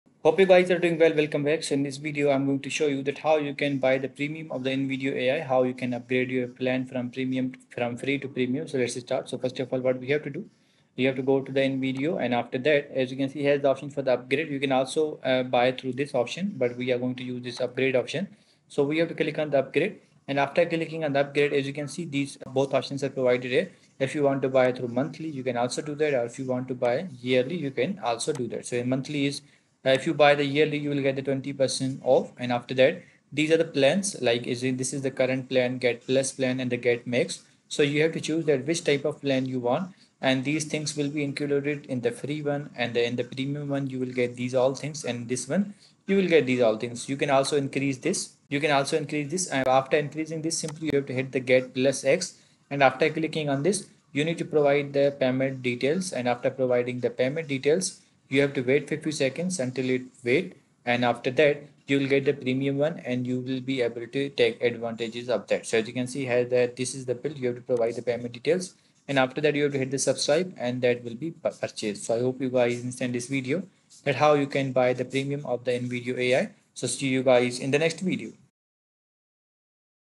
How to Buy Premium of the Invideo AI [easy]
Buying a premium subscription for Invideo AI is a simple process. Start by visiting the Invideo AI website and logging into your account. If you don’t have an account, you’ll need to create one first. Once logged in, navigate to the pricing or plans section, usually found in the main menu or footer of the site. Review the available premium plans and choose the one that best fits your needs. You will be prompted to enter your payment information, such as credit card details or an alternative payment method. After entering your payment details, review the subscription terms and confirm your purchase. Upon successful payment, your account will be upgraded to the premium plan, giving you access to additional features and benefits. Buying a premium subscription for Invideo AI is quick and easy, enhancing your video creation experience.